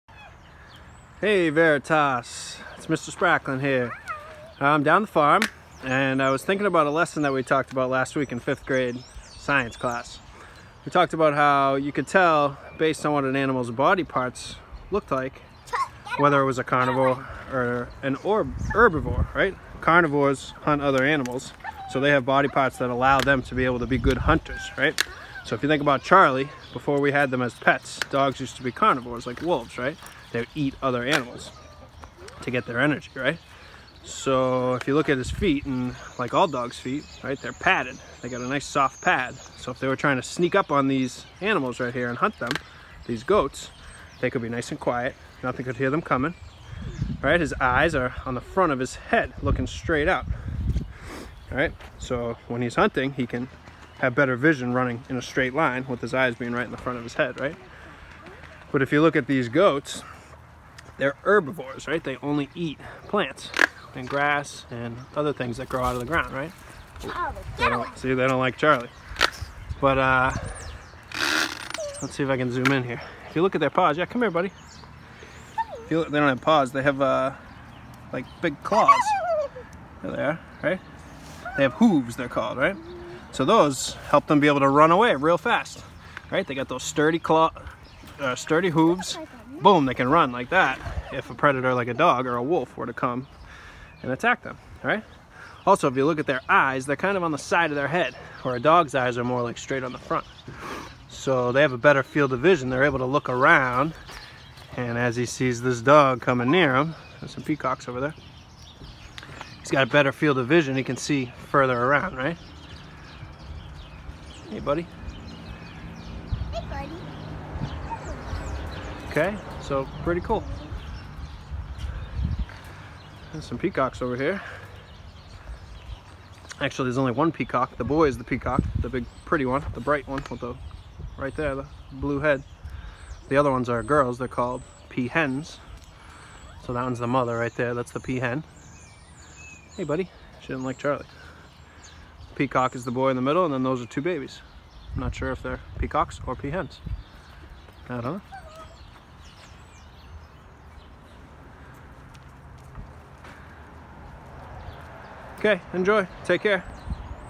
Body parts: carnivores vs herbivores
Compare the dog’s body parts (a carnivore) to the goats’ body parts (herbivores). Enjoy!